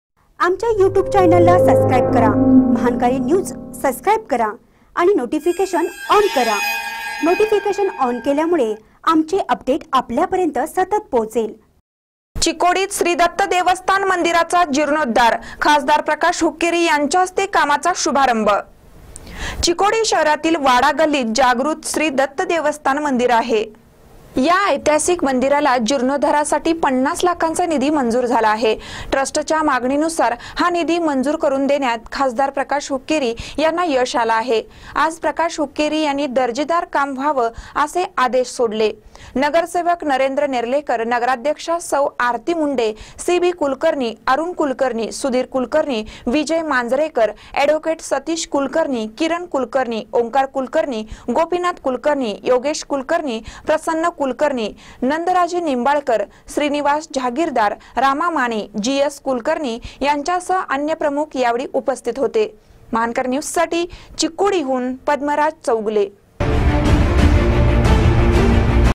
चिकोडीत श्री दत्त देवस्थान मंदीराचा जिर्णोध्दार..
चिकोडीत श्री दत्त देवस्थान मंदीराचा जिर्णोध्दार.. खासदार प्रकाश हुक्कीरे यांच्या हस्ते कामाचा शुभारंभ